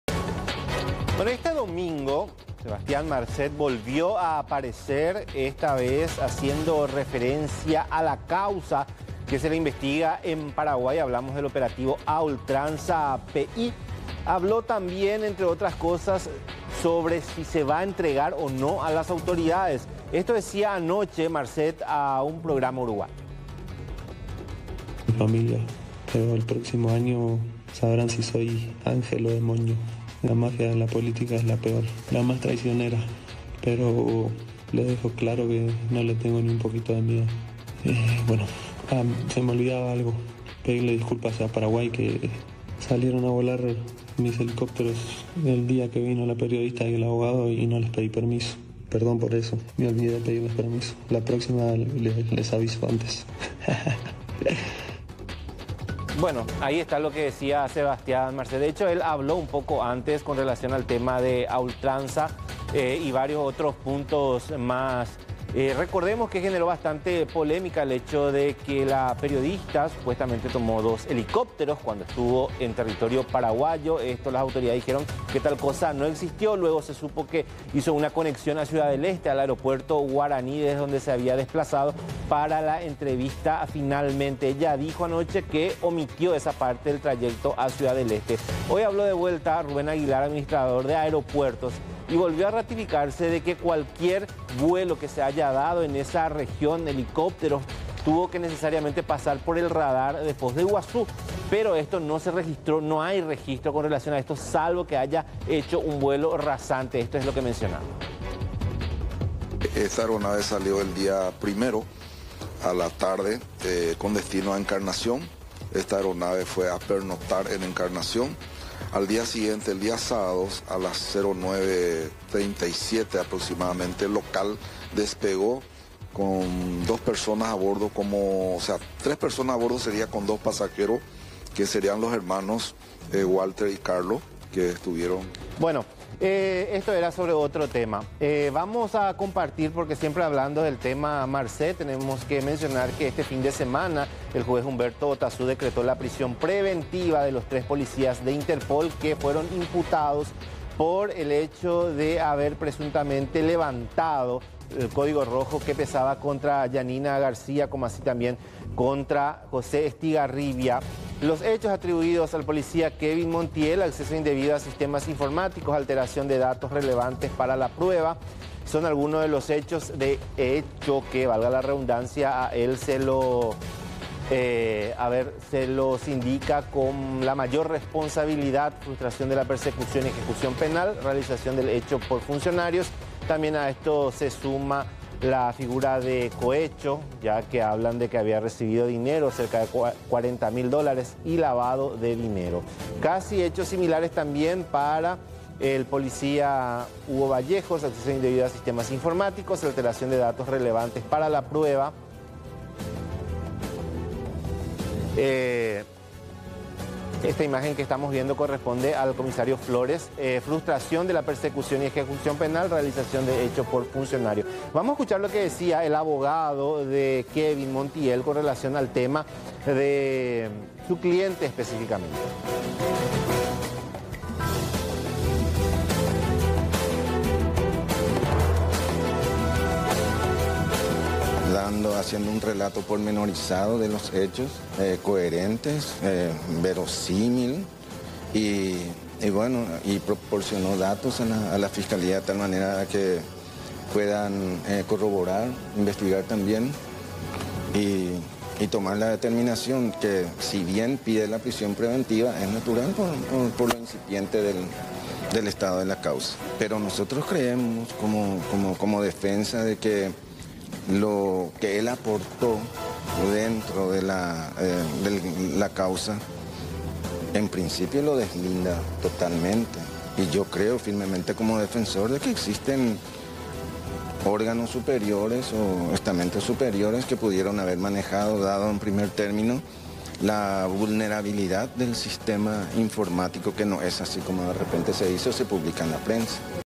Prisión para los policías de INTERPOL
Tras el escándalo del levantamiento del código rojo de la INTERPOL, se dio prisión para los policías involucrados